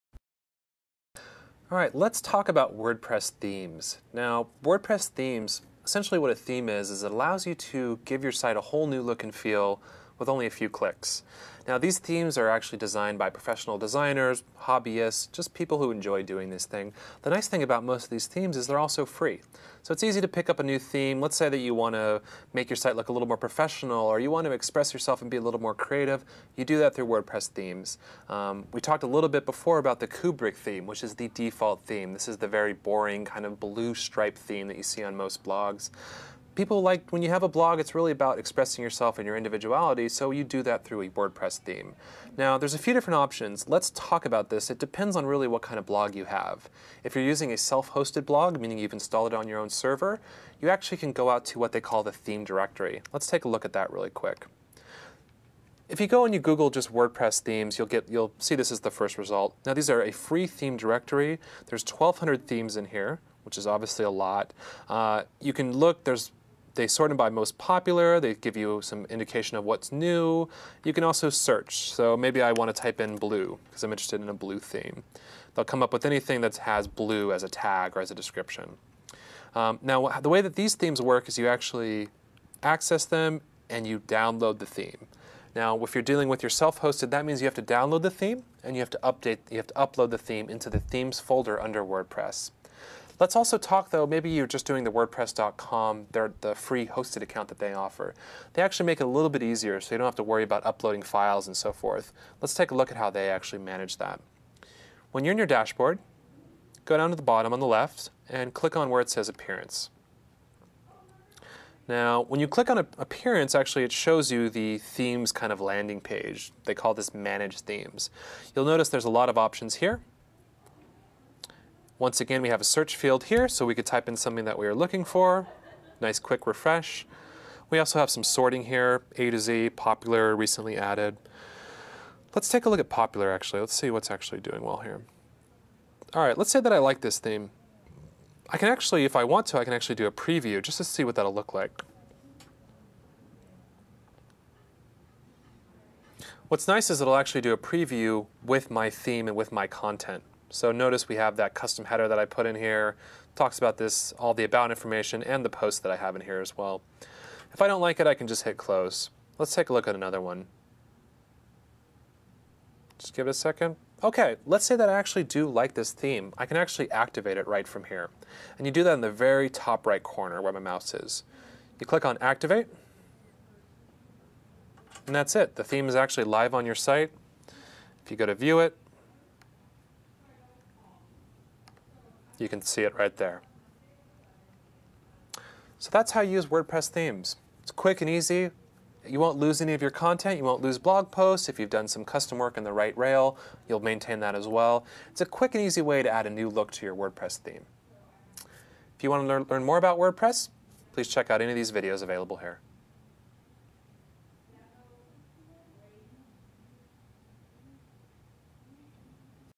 How To Change Your Theme In Wordpress
Sean Percival teaches you the basics of starting your own WordPress blog.

One of the best ways to draw in visitors and make your blog appealing is to customize its look and feel to reflect the subject that you are focusing on. If you want to customize your blog you will need to know how to change your theme in WordPress. Whether you are using WordPress's hosting services or if you are using the WordPress platform on your own webhost, changing the theme is a very easy process. There are over a thousand themes available, for free, and new ones are being added daily. With a few clicks of the mouse you will soon be able to make your blog look exactly how you want it to look.